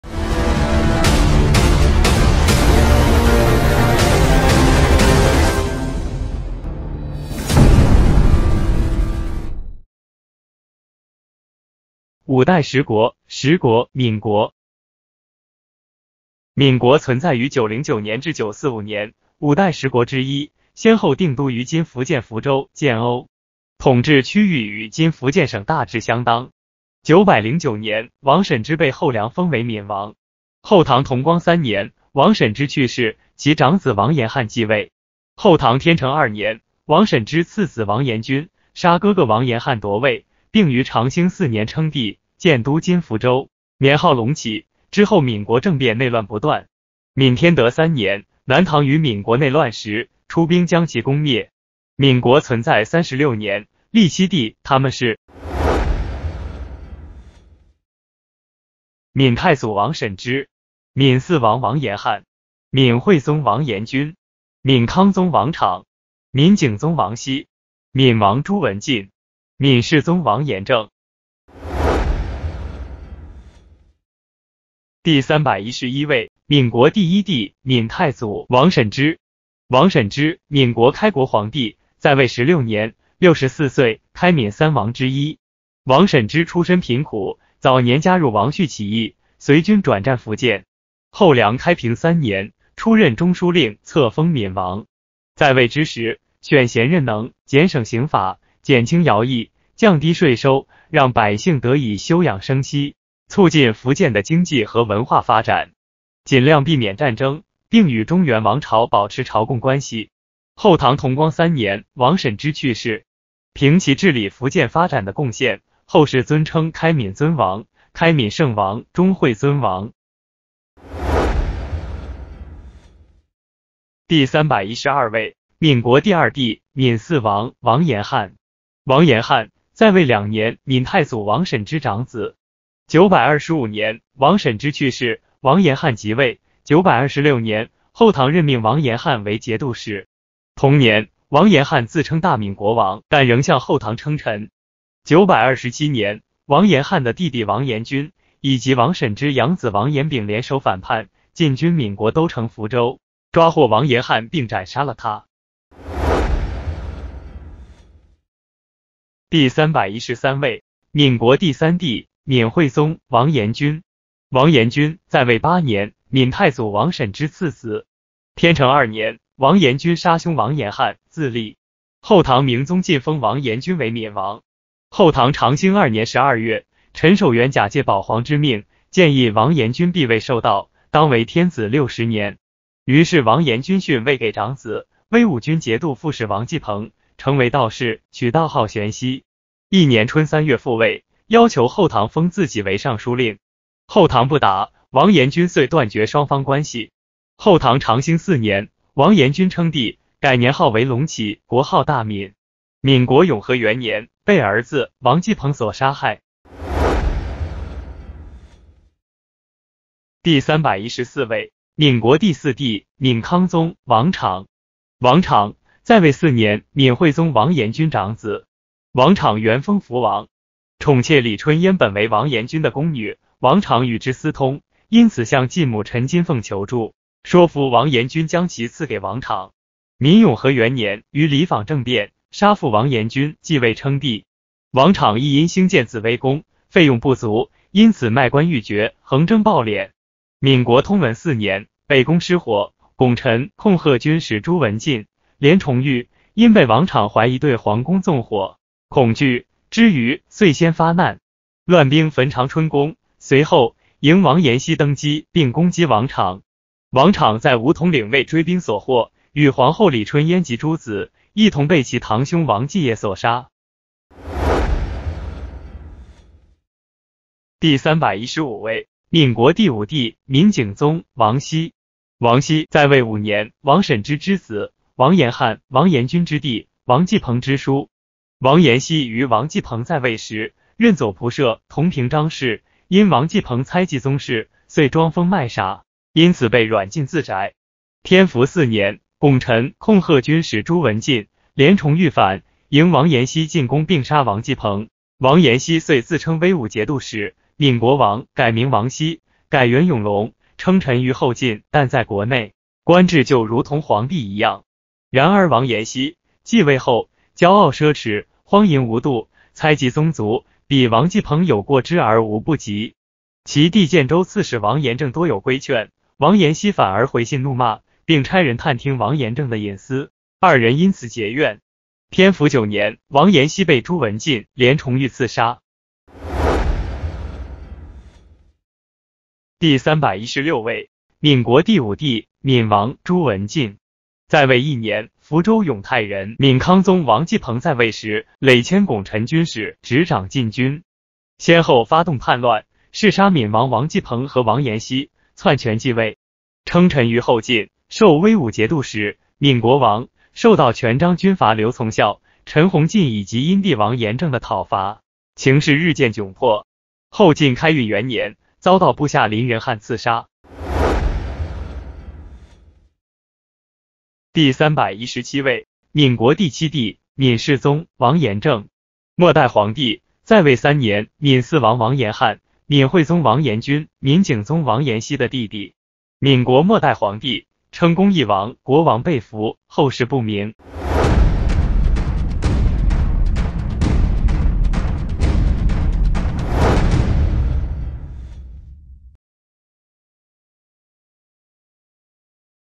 第50集 闽国 （王审知），909年—945年 50 MINGUO Dynasty · Complete Works of Chinese Emperors
00:00 中国朝代帝王全集：五代十国·十国·闽国，7帝 Emperors 7：
01:11 闽太祖·王审知
02:02 闽嗣王·王延翰
02:37 闽惠宗·王延钧
03:27 闽康宗·王昶
04:24 闽景宗·王曦
05:27 闽王·朱文进
06:01 闽世宗·王延政